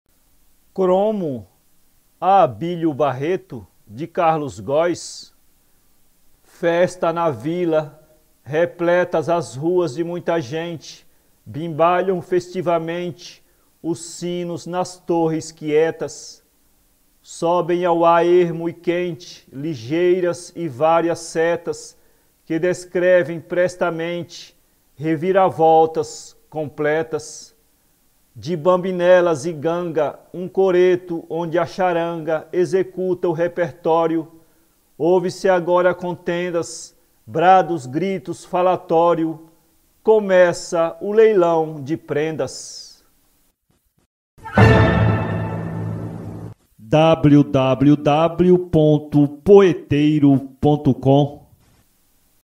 Cromo (Poema, 1910), de Carlos Góes
Carlos Góes foi um gramático, filólogo, teatrólogo e poeta. Como poeta escreveu "Crótalos", a que seguiu "Cítara".